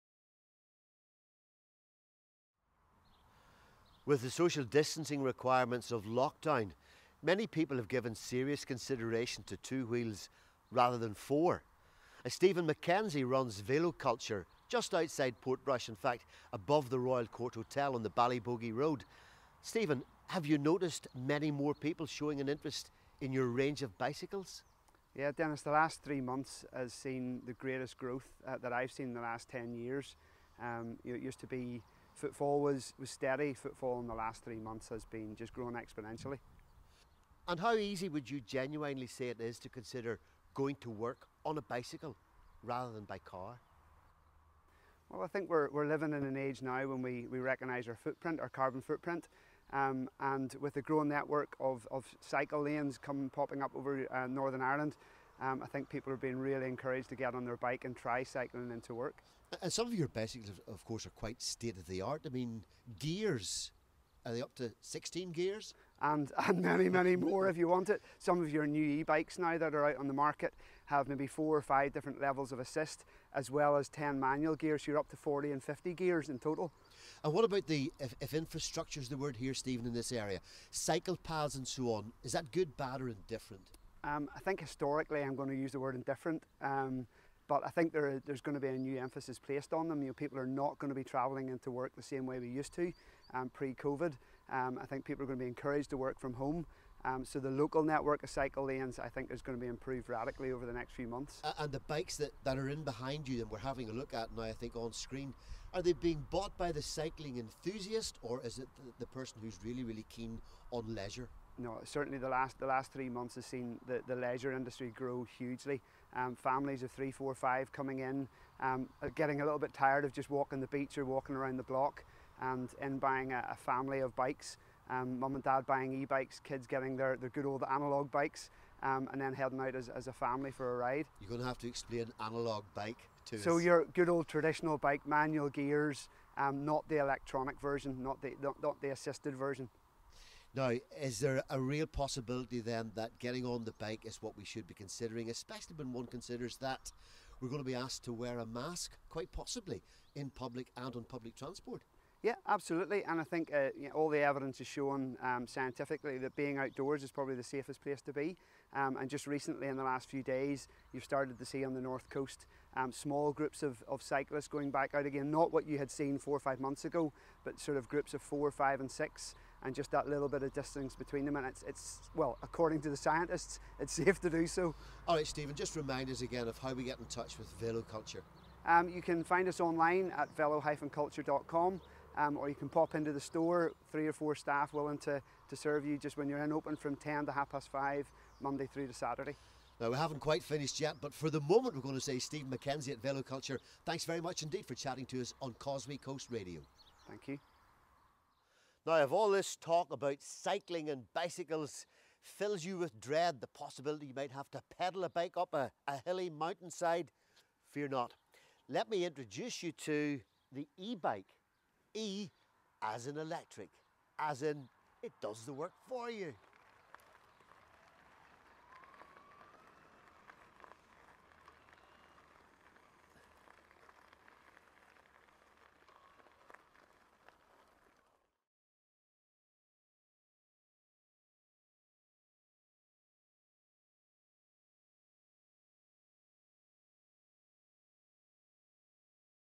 Causeway Coast Radio - Denis McNeill chats to Stephen McKenzie of Velo Culture
Denis visits Velo Culture just outside Portrush and talks to Stephen McKenzie about bikes and the massive increase in interest for the two wheel mode of transport.

Velo Culture